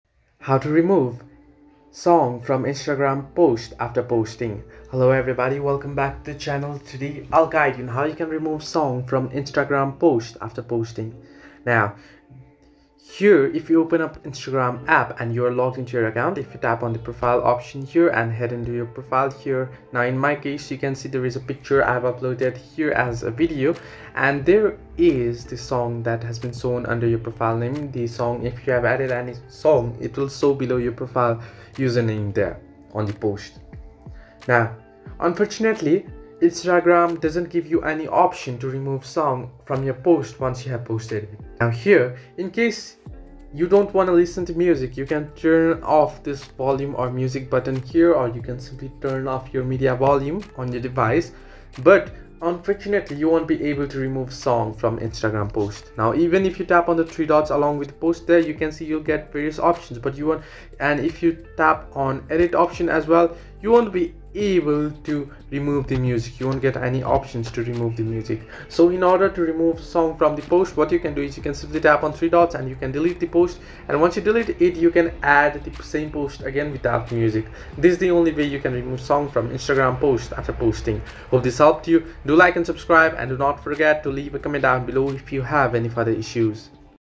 How To Remove Song From Instagram Post After Posting !
In this tutorial video, I will simply show you how to remove song from Instagram post after posting. So just make sure to watch this video till the end.

~ Video Chapters:
0:00 Introduction
0:11 Overview & Guide
1:17 Outro